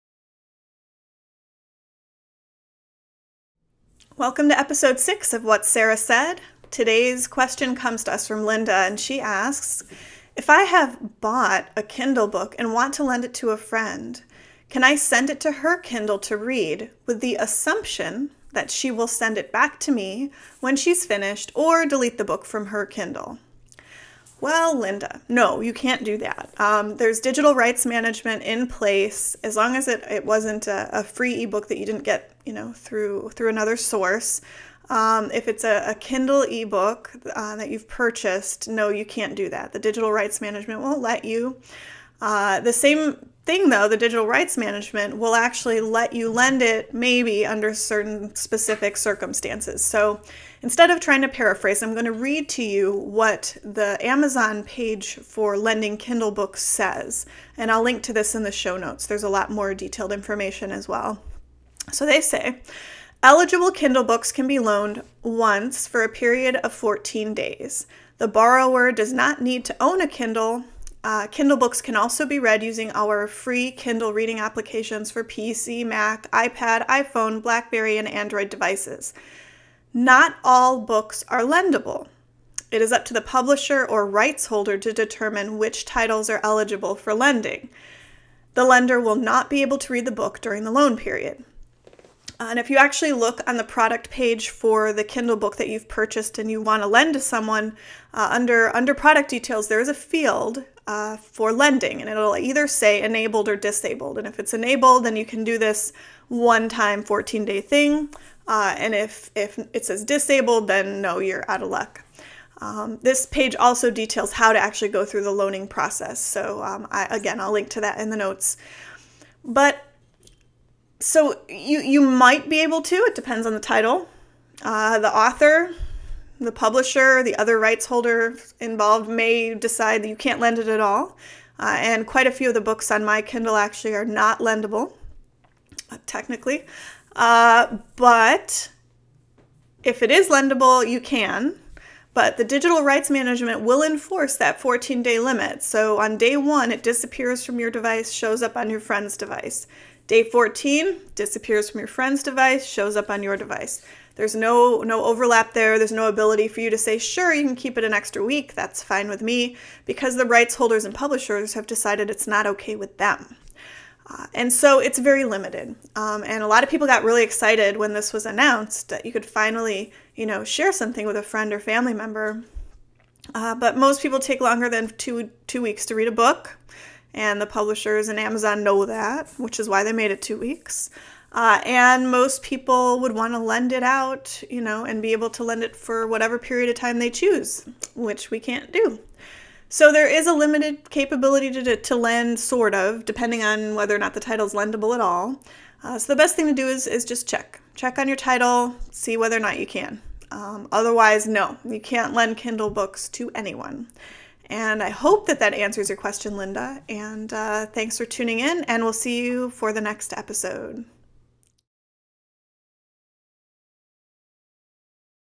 What Sarah Said 6: Episode 6: Sharing Kindle eBooks
Sarah Houghton (Librarian in Black) answers a question from Linda about whether or not you can loan a Kindle book to a friend with the assumption he or she will return it when finished. Bonus cat shenanigans in the background ;)